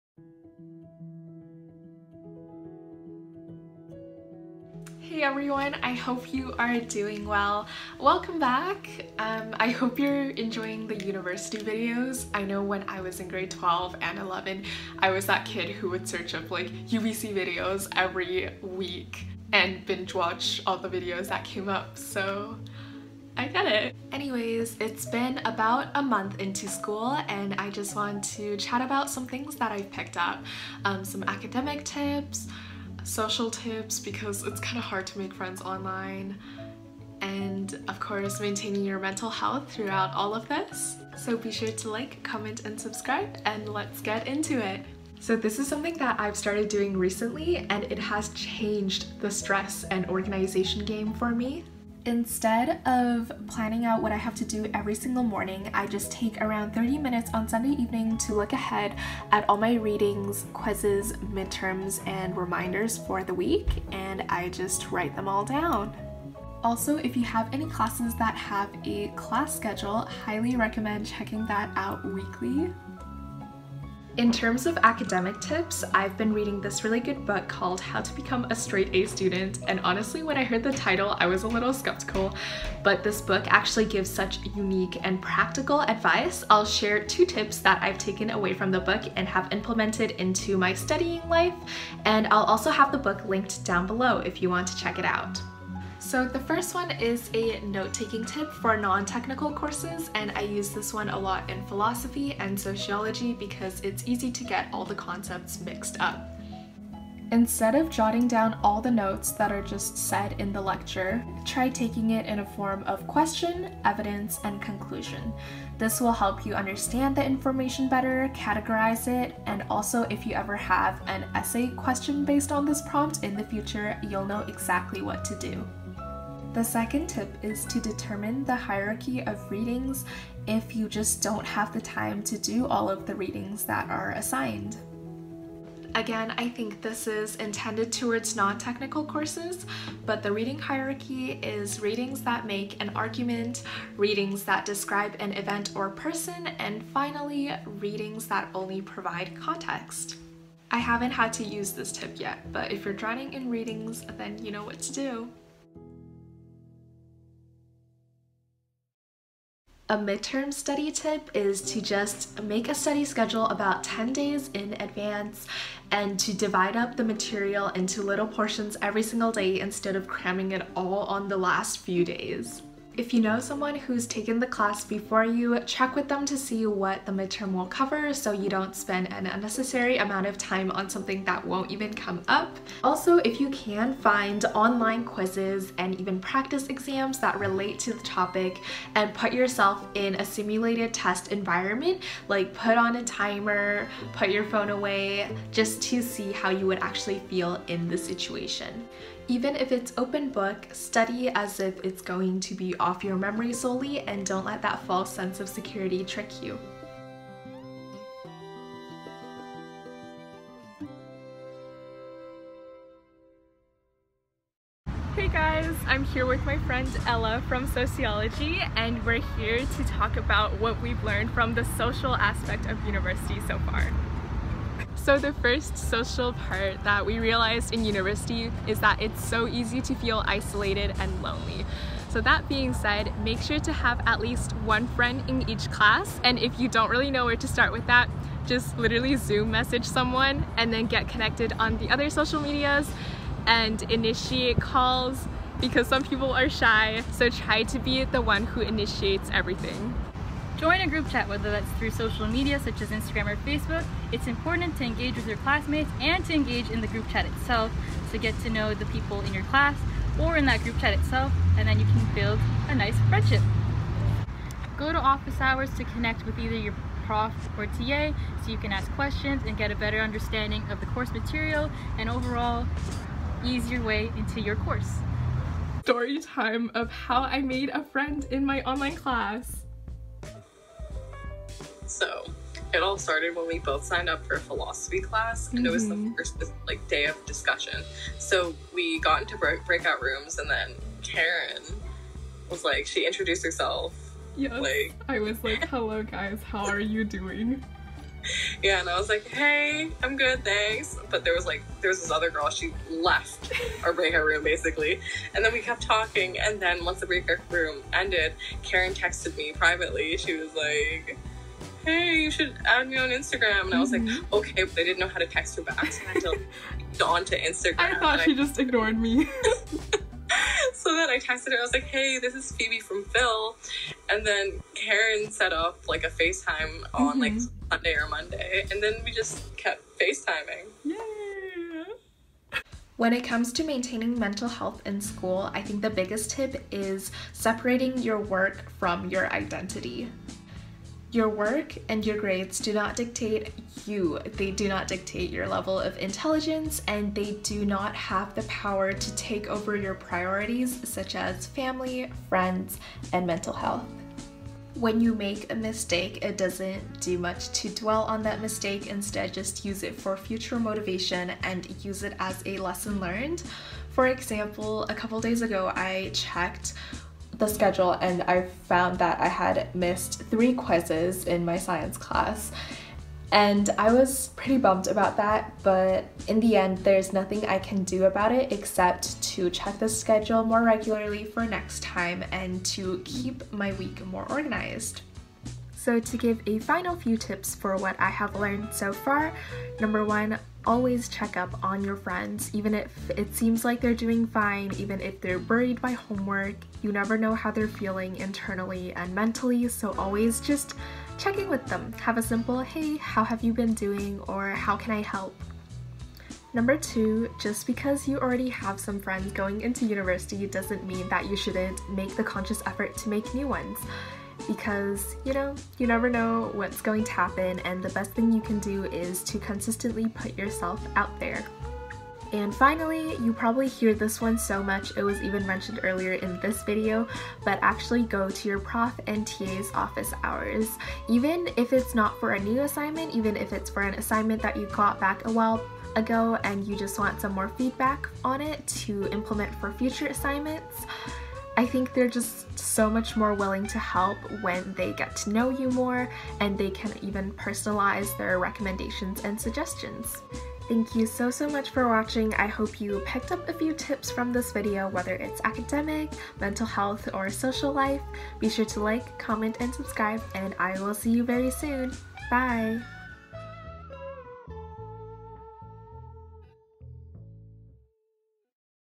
10 UNIVERSITY TIPS I’VE LEARNED SO FAR | UBC
So, I've been officially studying at the University of British Columbia for a month... here are 10 lessons that I have learned so far! Academic, midterm study tips, social life, mental health, it's all compiled here! Also happy Thanksgiving hehe x

Outtro by @mundeign on Instagram.
Intro drawing by @alyssadoesthe_art on Instagram.

Have a magical, wonderful, fantastic day!